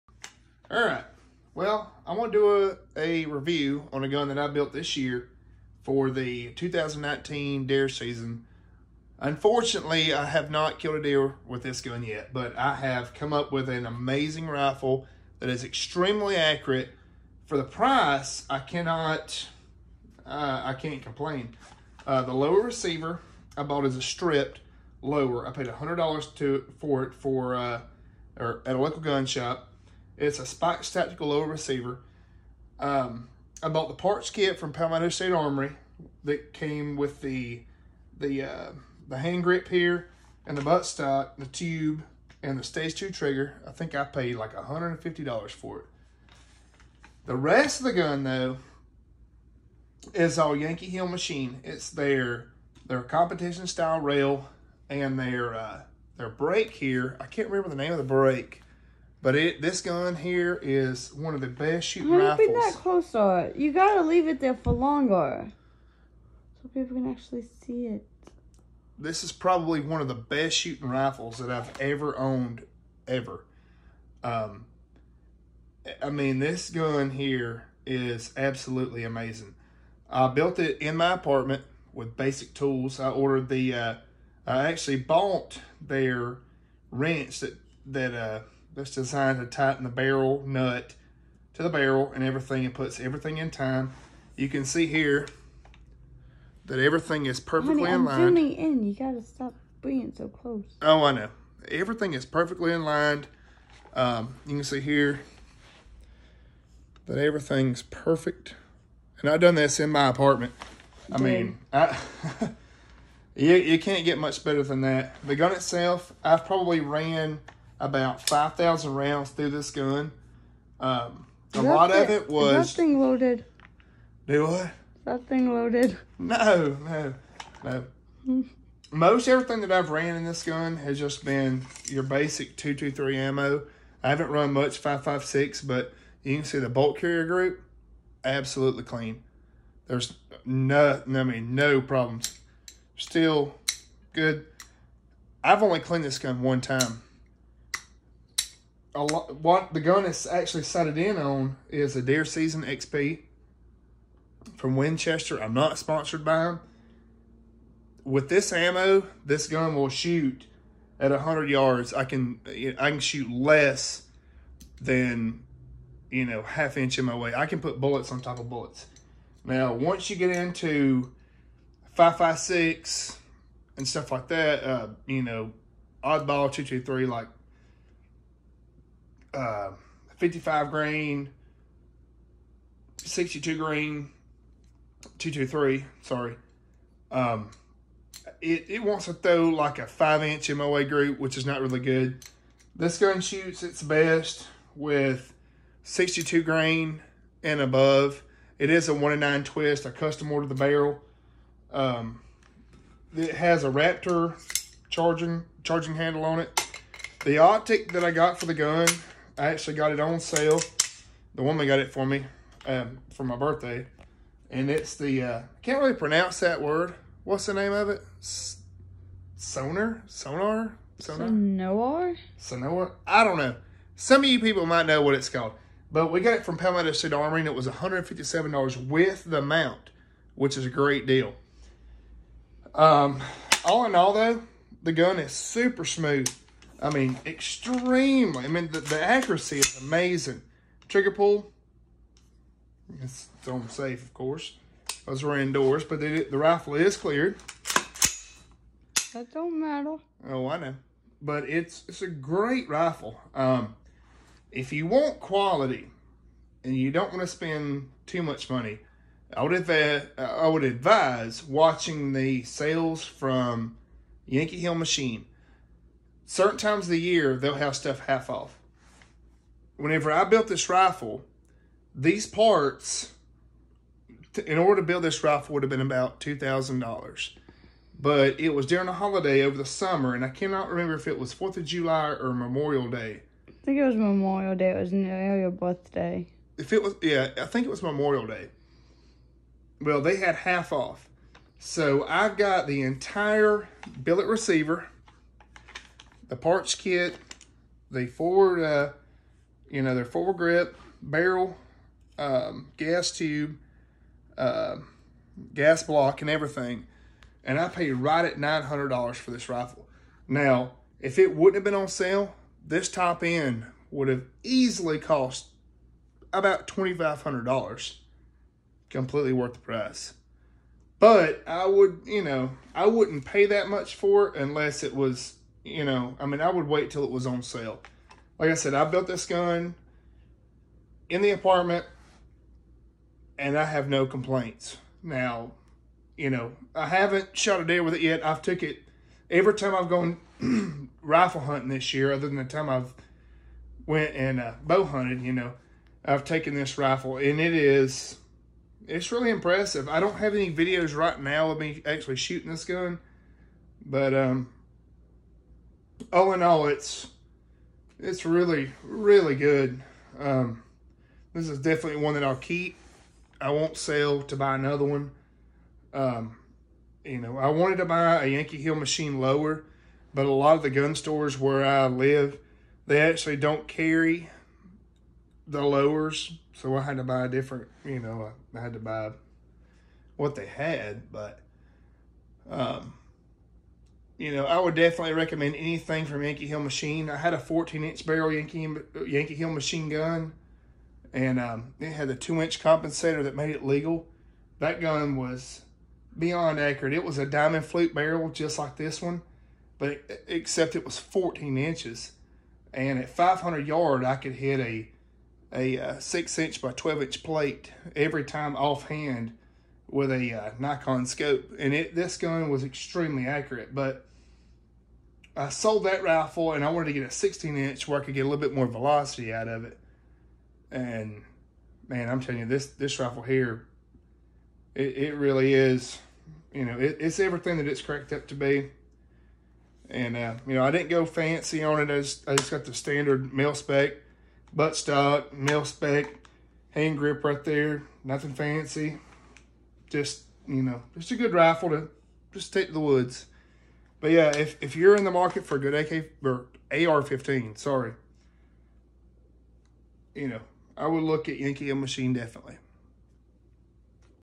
Yankee hill machine review
This raffle is a Forged Yankee Hill machine top end And a Spikes tactical lower with Palmetto State Armory internals.